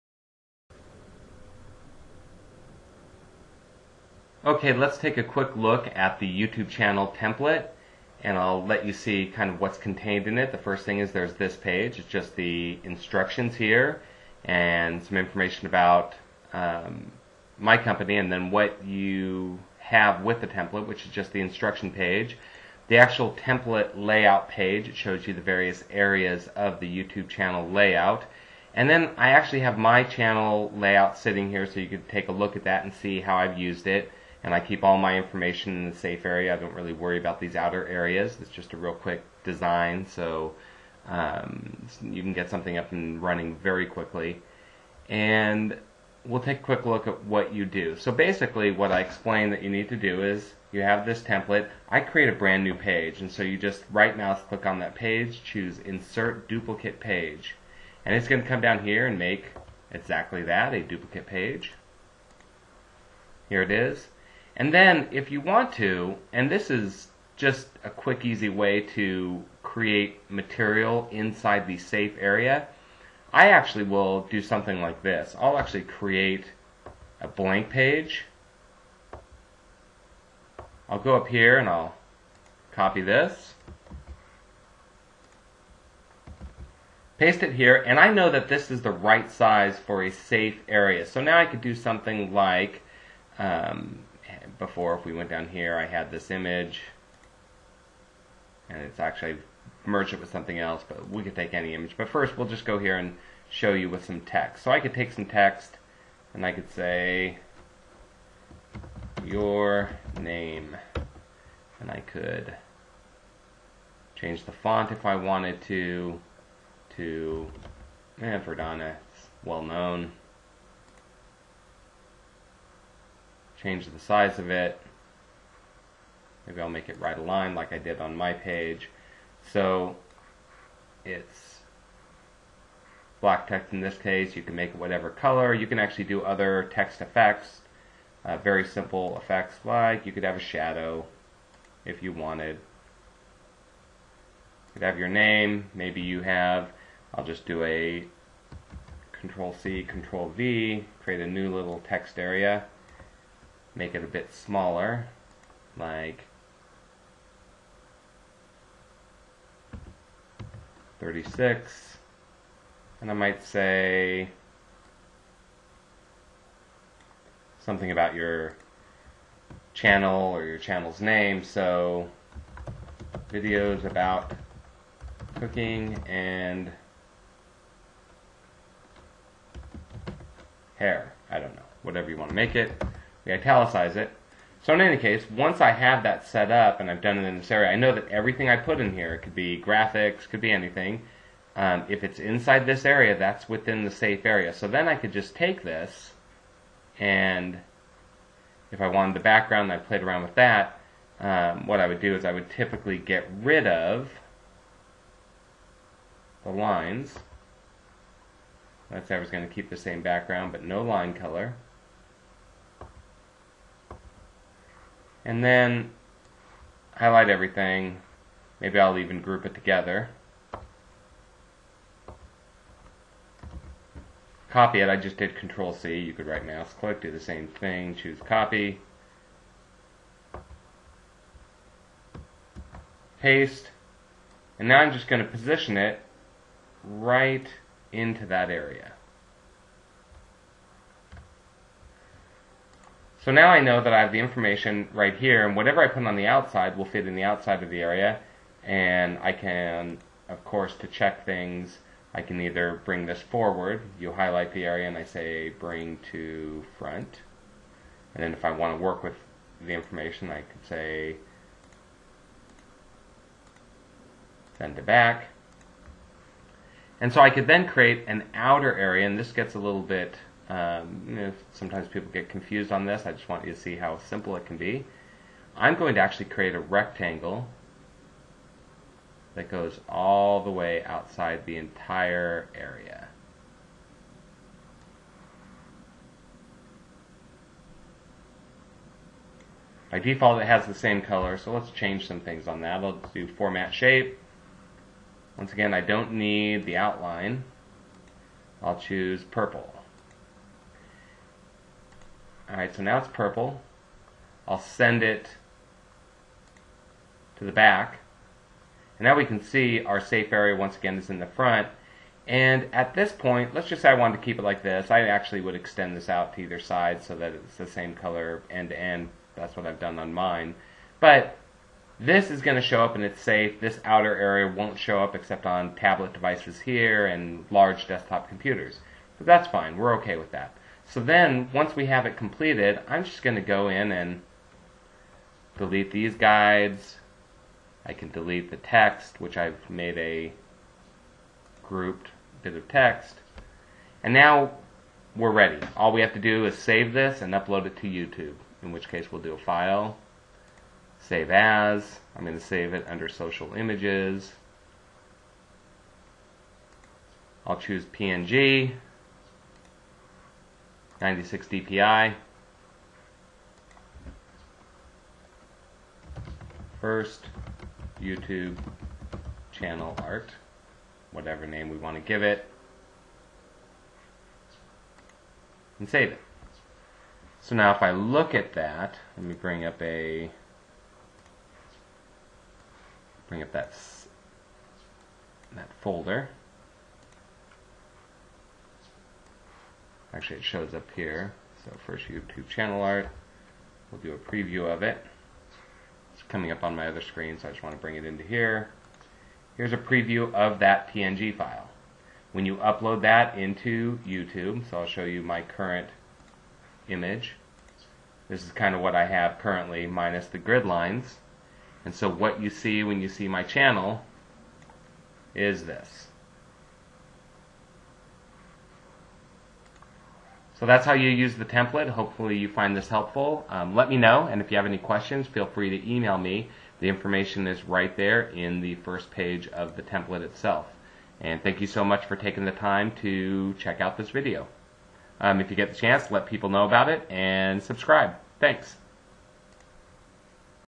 YouTube Channel Art Template in Microsoft Publisher
This video shows how to use my YouTube Channel Art template for Microsoft Publisher. You can download the template at: